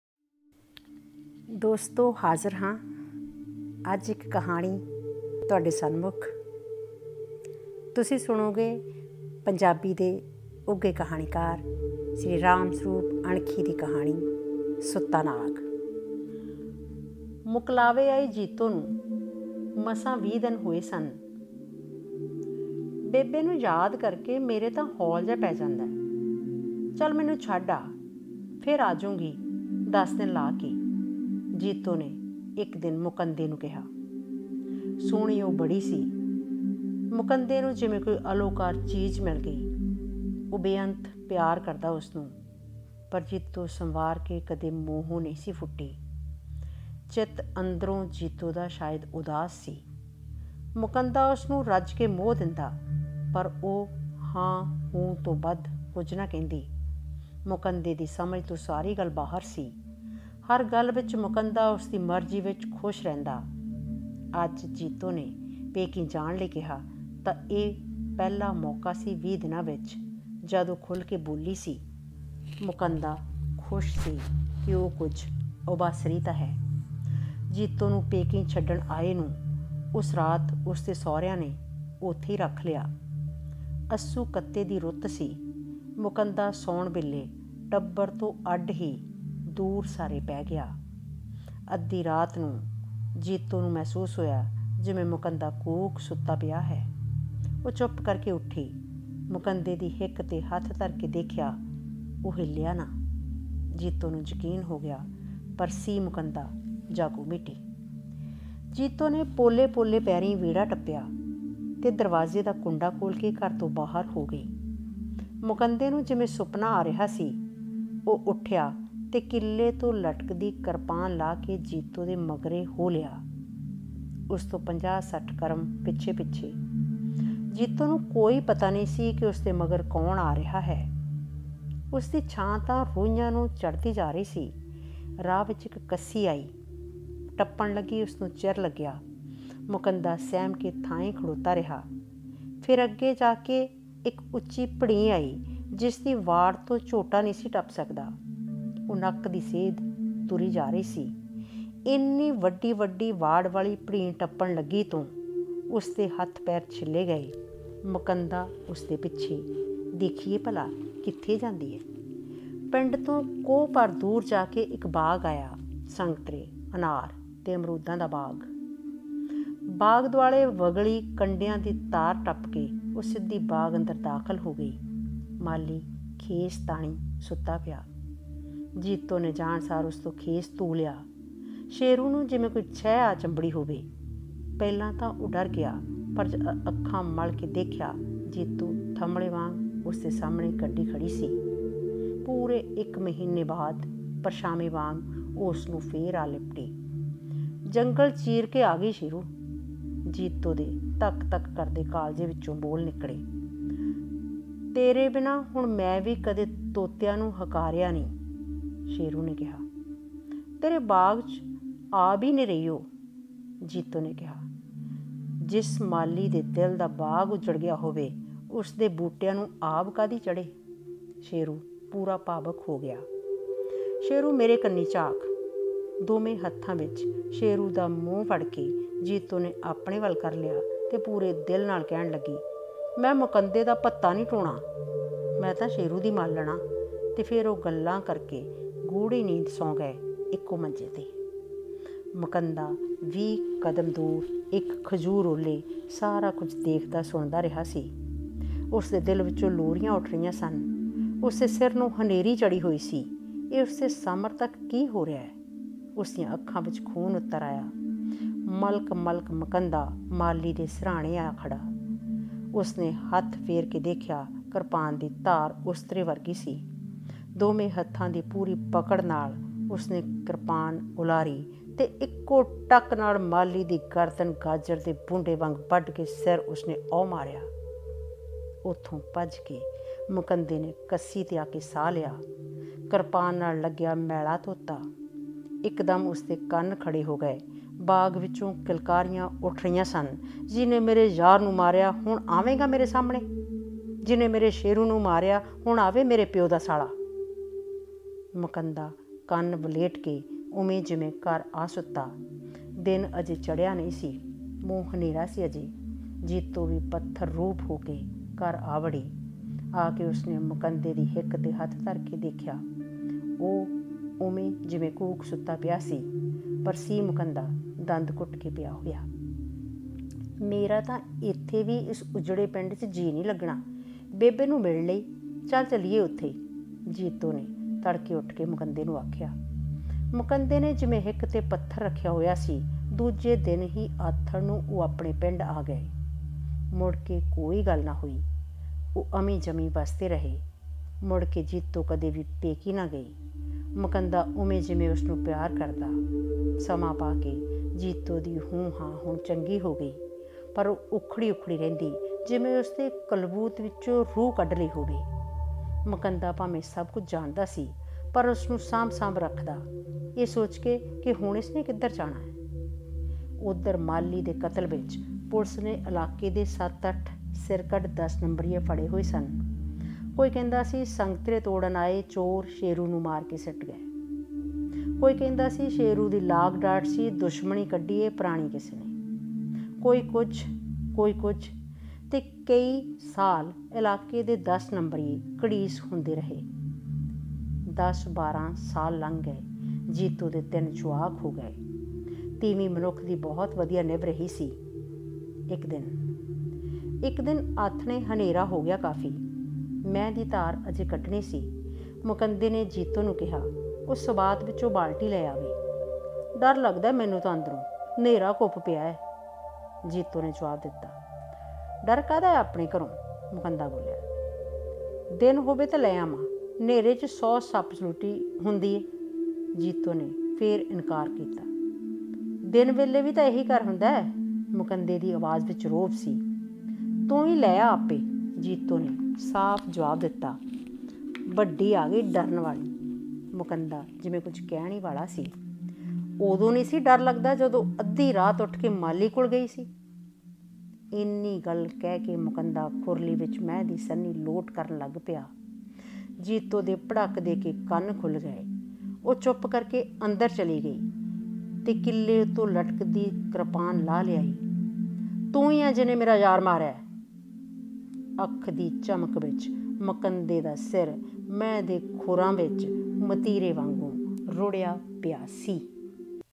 ਸੁੱਤਾ ਨਾਗ - ਰਾਮ ਸਰੂਪ ਅਣਖੀ | Sutta Naag (Punjabi Short Story) AudioBook- Ram Sarup Ankhi
ਸੁੱਤਾ ਨਾਗ- ਰਾਮ ਸਰੂਪ ਅਣਖੀ
ਪੰਜਾਬੀ ਕਹਾਣੀ
Ram Sarup Ankhi was one of the prominent Punjabi writers of all time, who mastered the art of writing Punjabi Short Stories. Though he is read by the individuals of all ages, and is most popular among the youth.

His stories celebrate the unsung shades of Indian Society, specifically Punjab. The desires which are butchered in the name of norms and traditions, The writers talks about them fearlessly.

Short story- Sutta Naag
Writer- Ram Sarup Ankhi

Voice- Dr. Ruminder Johal